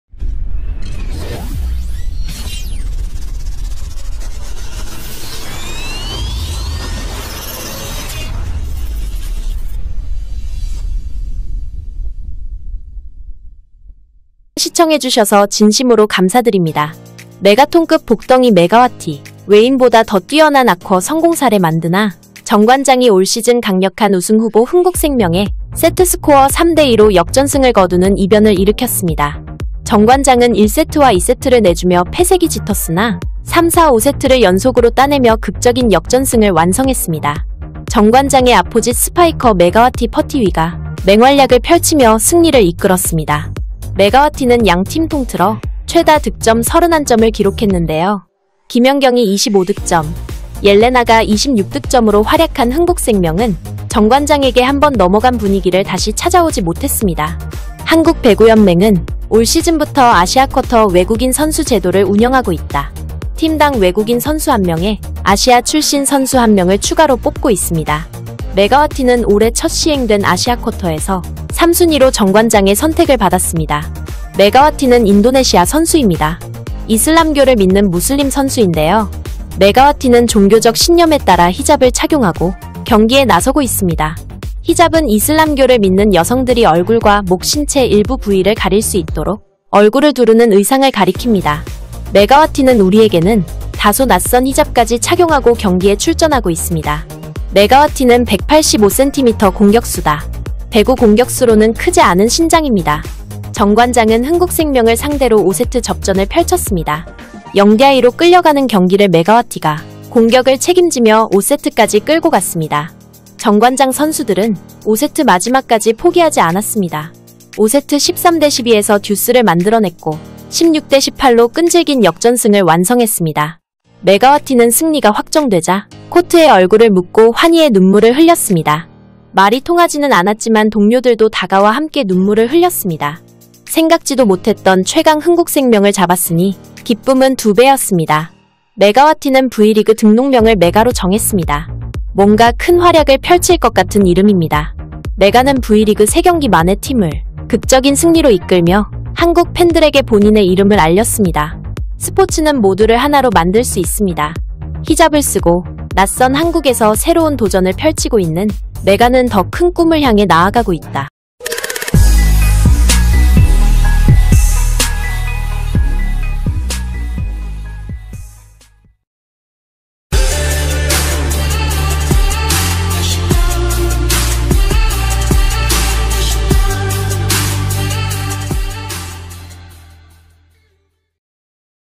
MEGATRON MEMIMPIN KEMENANGAN DRAMATIS ATAS JUARA BERTAHAN PINK SPIDER || RED SPARKS @Sports mania
Sports Lover adalah channel Sports diberbagai cabang olahraga dan beritanya sedang hangat dibicarakan dan viral dimedia massa.